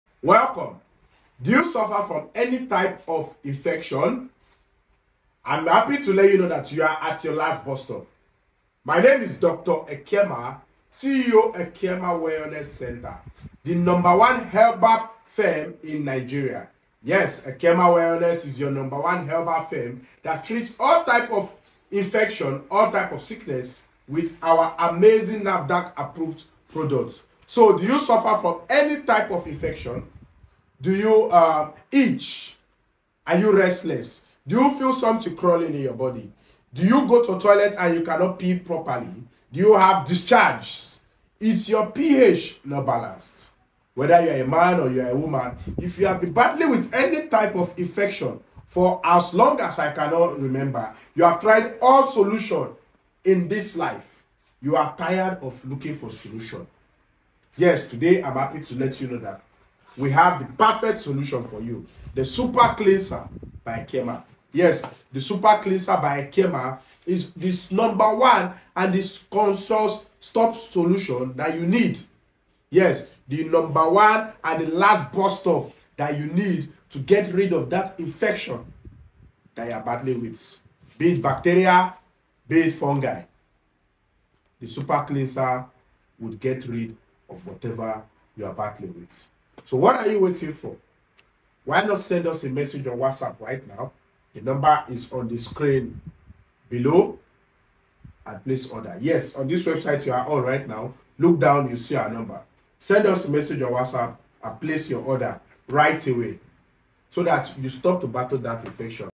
DR EKIEMA TALKS ABOUT THE EFFICIENCY OF SUPA CLINSA
The Supa Clinsa by Ekiema helps fight of BACTERIA infections of any kinds.